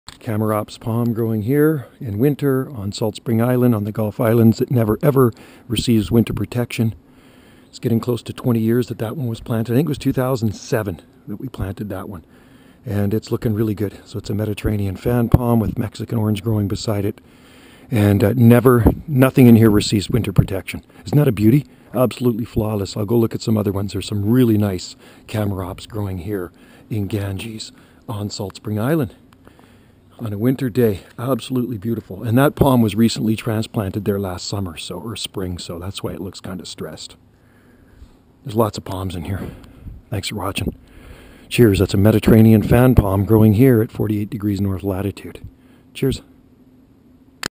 No Winter protection needed for Chamaerops Palm trees on the Gulf Islands
Chamaerops humilis palm in Winter on  Salt Spring Island